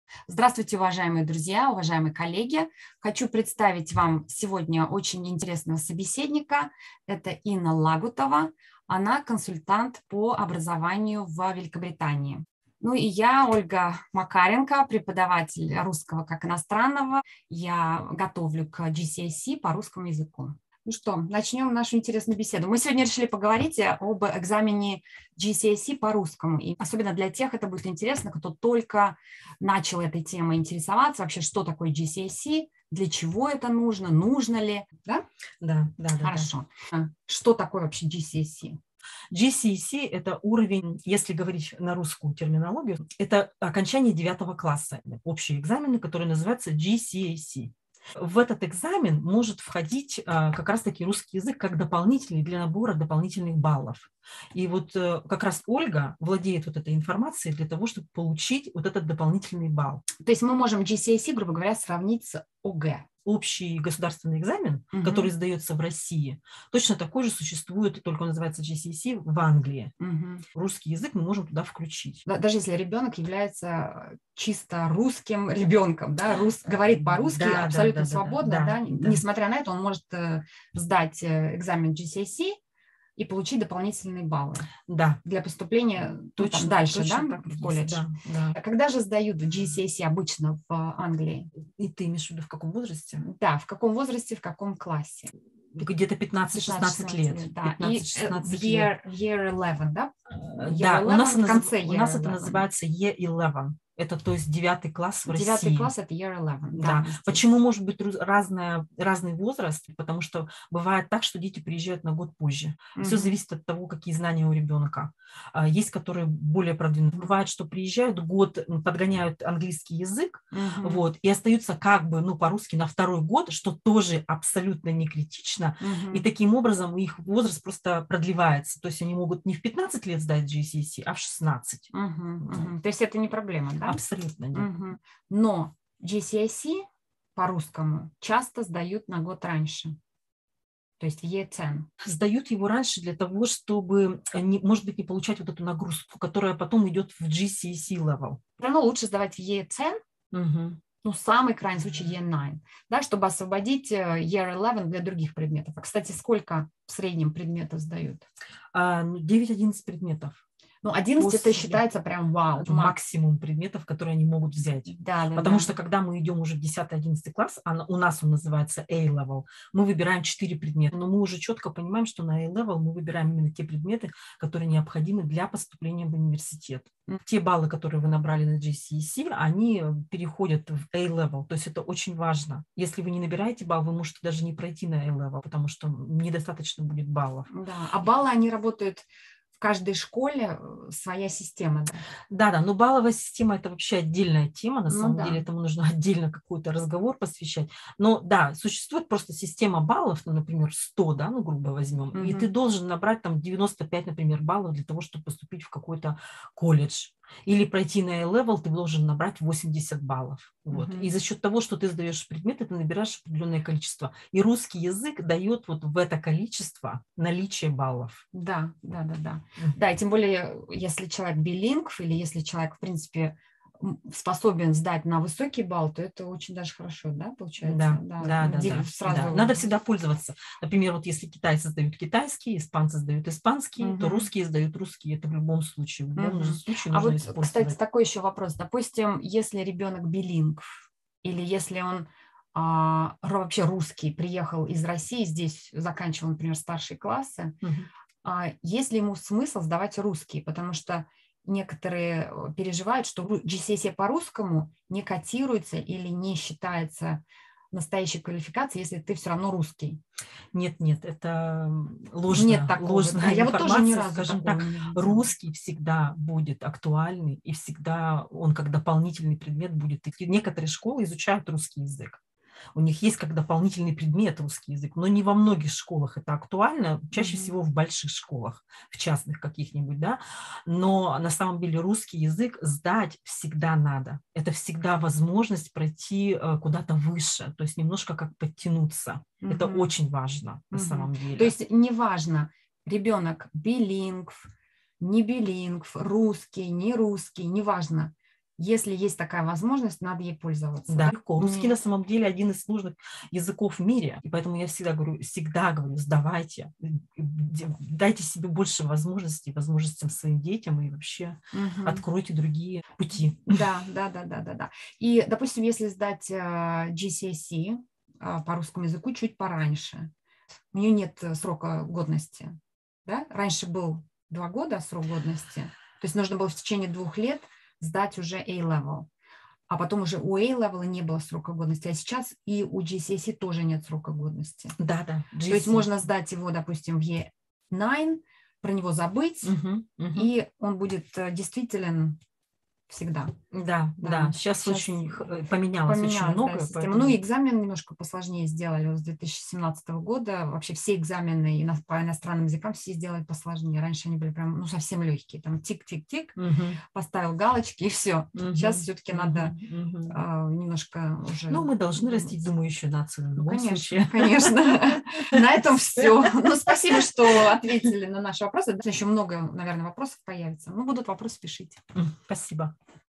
Your questions answered - GCSE Russian
This video may be of interest to someone who’s just entering the realm of GCSE exams and they need to familiarise themselves very briefly about it. The video was made in association with Inna Lagutova - Education consultant specialising in the UK education system.